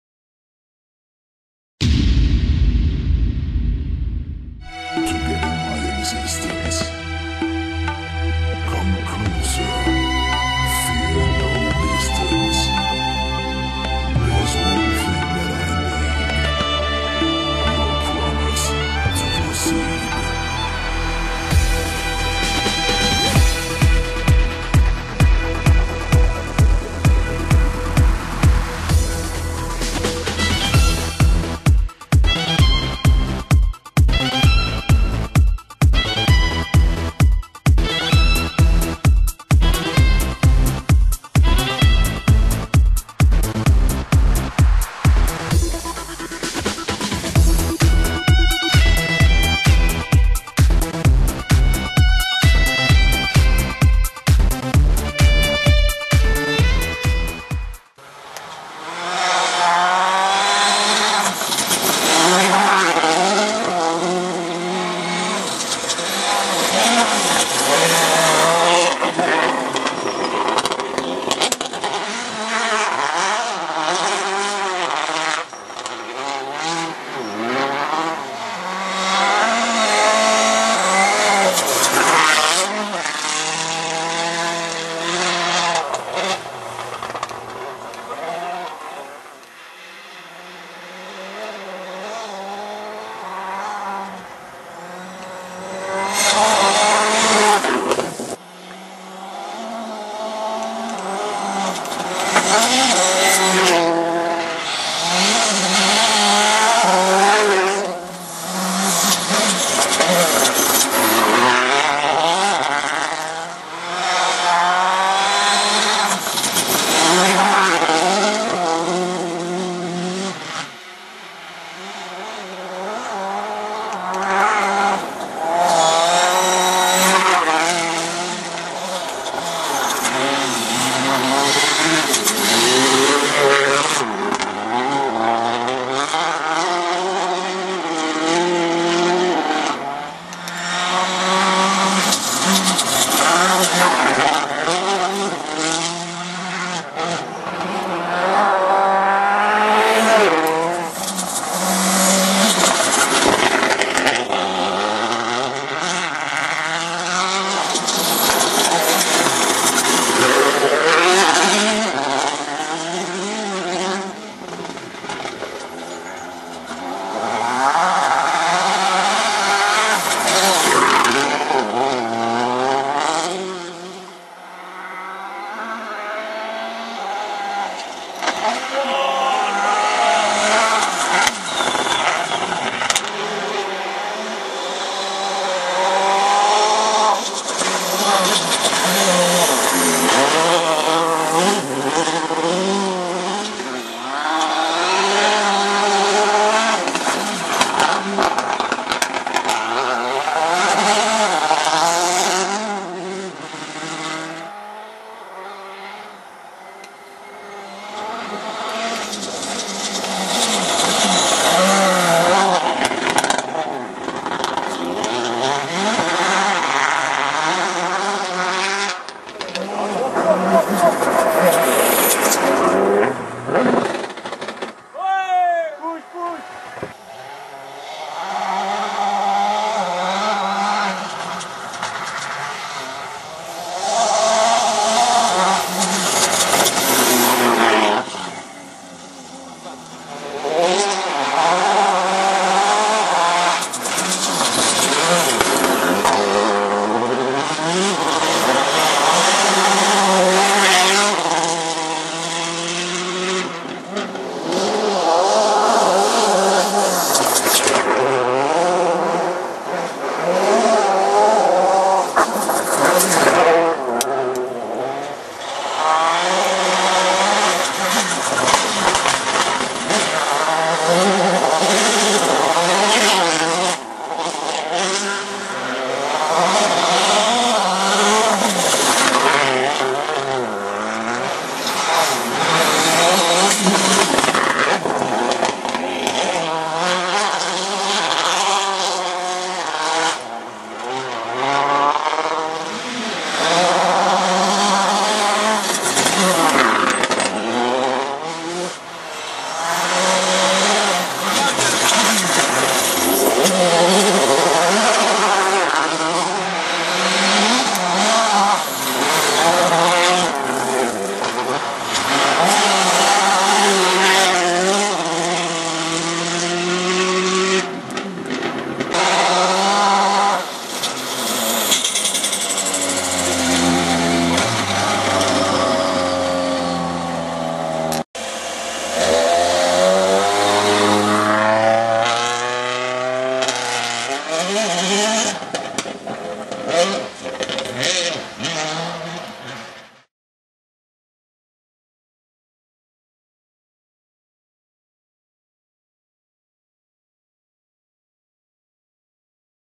Sebastien loeb tests terre.
Essais de Seb et Daniel le 17 et 18 Novembre 2009 proche de Cardona, Catalogne.